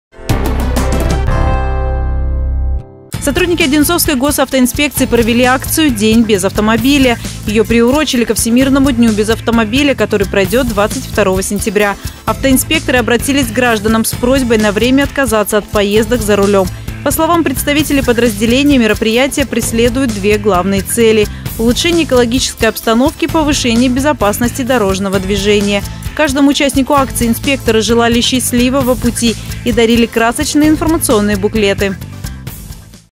День без автомобиля
Сотрудники Одинцовской Госавтоинспекции провели акцию «День без автомобиля».